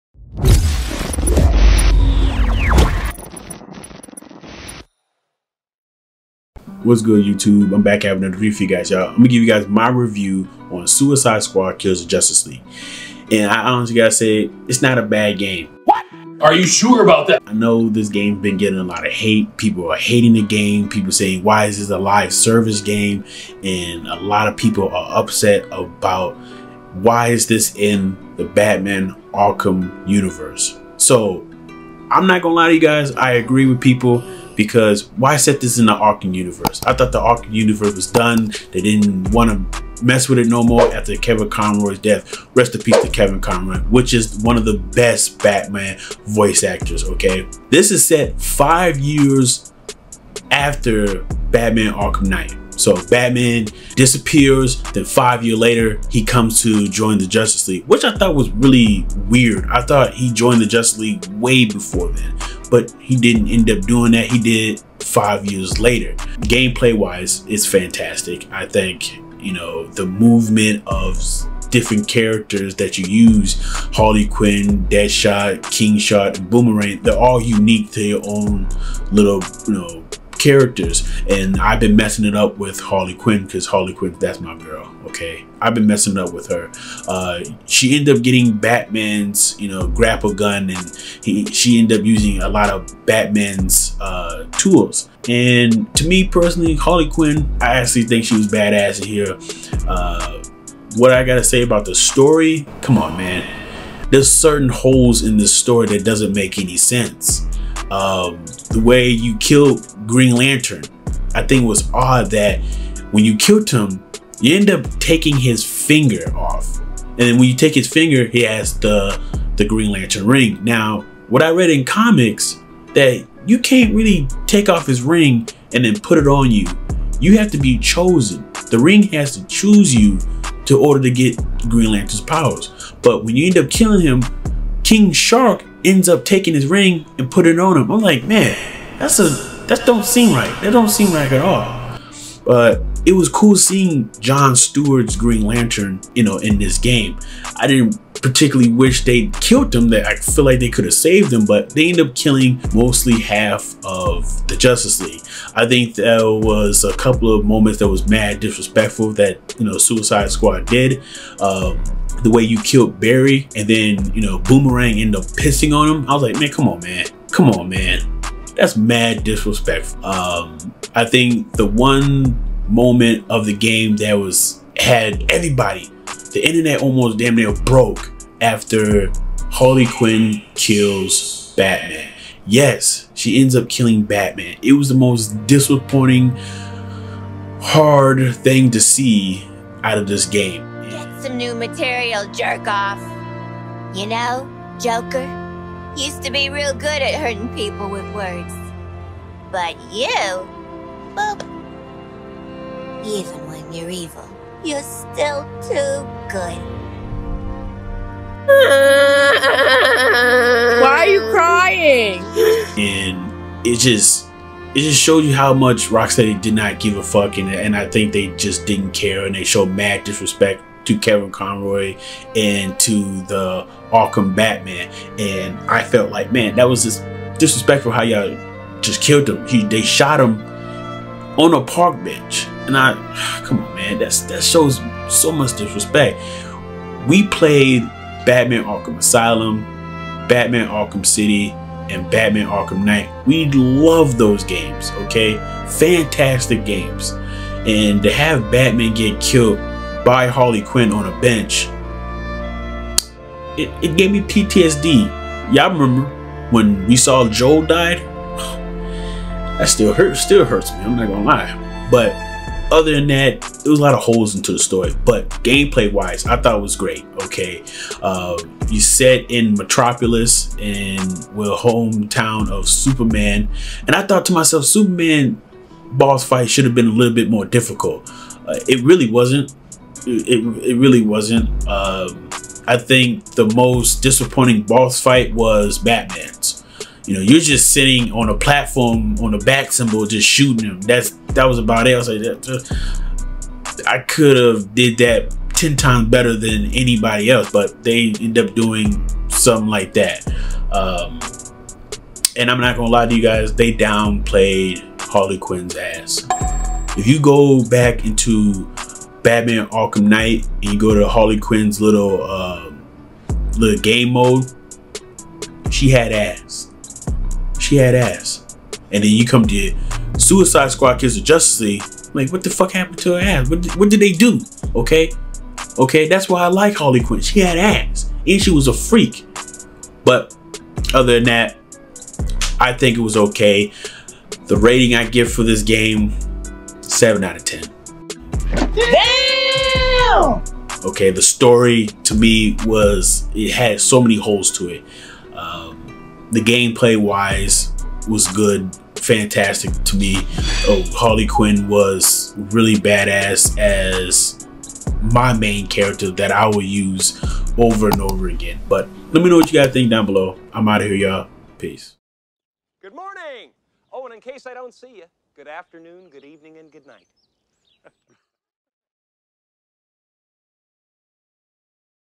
Suicide squad kills the Justice league is not a bad game So calm down
This is my honest review of this game